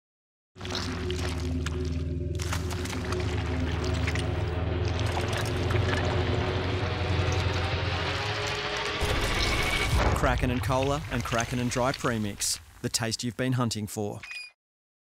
The Kraken Black Spiced Rum | Now Available in Premix
Bar quality at home is within your grasp this summer with Kraken & Dry and Kraken & Cola available for the truly dedicated to claim for themselves.

The time has come to enjoy bar quality at home with the new range of Kraken premix. Long desired by those truly dedicated to their pursuit of the beast, keep a keen eye out for the Kraken premix range on shelves at all good liquor stores.